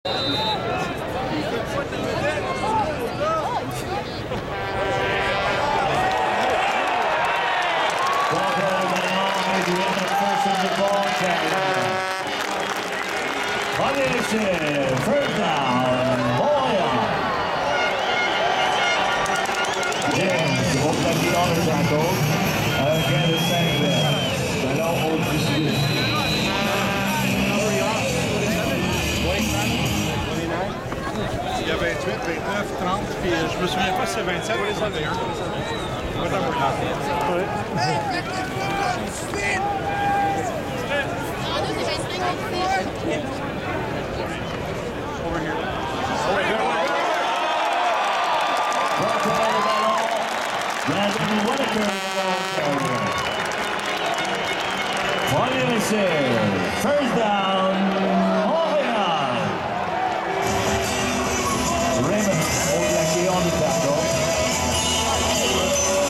Alouettes against Calgary
We watch when Alouttes plays against Calgary